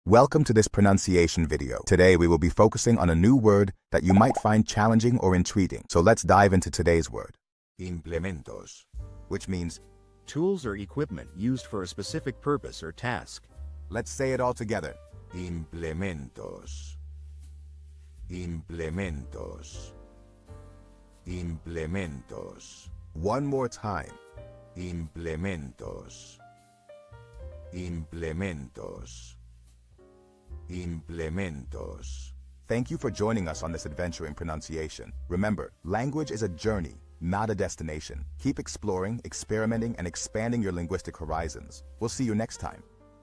How to pronounce Implementos
Master the Pronunciation of 'Implementos which means implements' - which means : Tools or equipment used for speech Purpose Or Task. 📝 with @PronunciationSpanish 🎓🗣️ - [Your Guide to Spanish]

Learn to pronounce 'Implementos' perfectly in Spanish with our easy and engaging tutorial by @PronunciationSpanish! 📖📽️ In this comprehensive video, we provide you with native speaker examples 🗣️👂 and phonetic breakdowns 🎵 to ensure you get the pronunciation just right.

How to say Implements in Spanish